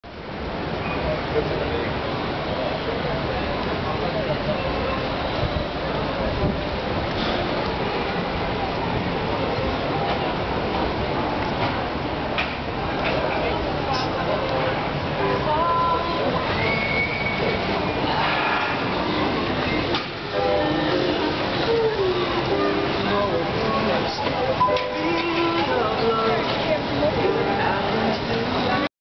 Vienna-Ferris Wheel
Summer 2007
Jack Fitzpatrick and Greg Weckenbrock
We rode here in small tribute to "Before Sunrise"
-I had to tell Jack this was right on our way... it was a lie.